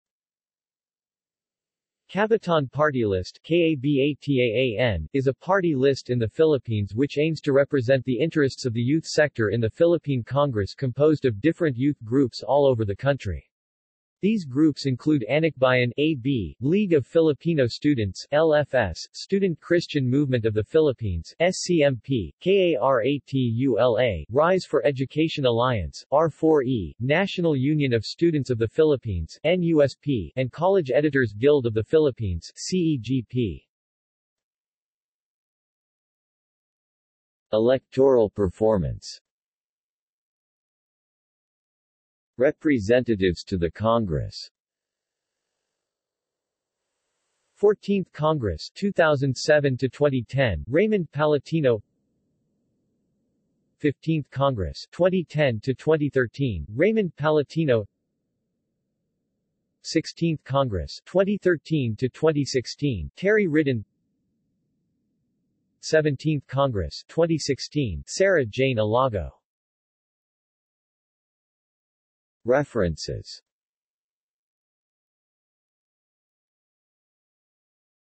Kabataan | Wikipedia audio article
This is an audio version of the Wikipedia Article:
Kabataan

SUMMARY
Kabataan Partylist (KABATAAN) is a Party-list in the Philippines which aims to represent the interests of the youth sector in the Philippine Congress composed of different youth groups all over the country. These groups include Anakbayan (AB), League of Filipino Students (LFS), Student Christian Movement of the Philippines (SCMP), KARATULA, Rise for Education Alliance (R4E), National Union of Students of the Philippines (NUSP) and College Editors Guild of the Philippines (CEGP).